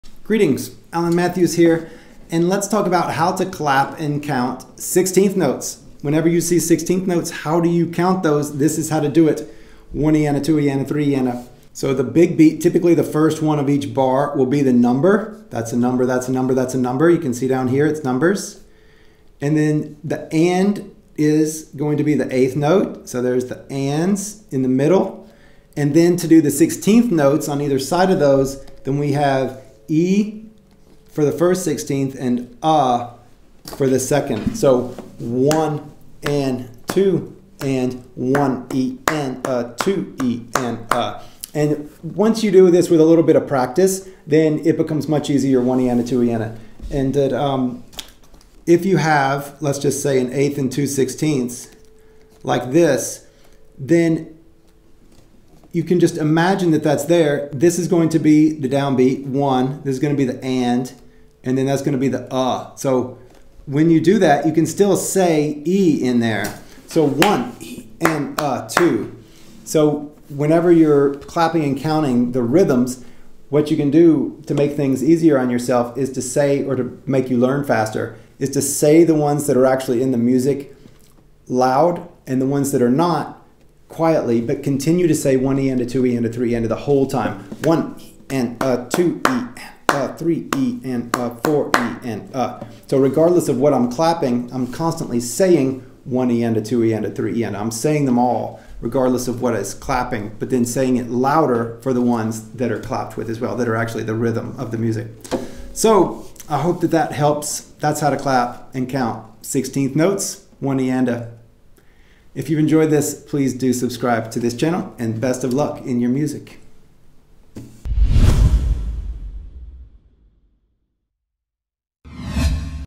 How to Count 16th Notes
Discover how to count 16th notes. Get the rhythm right, and everything you play will sound and feel better.

For a more complete exploration of clapping and counting music rhythms, go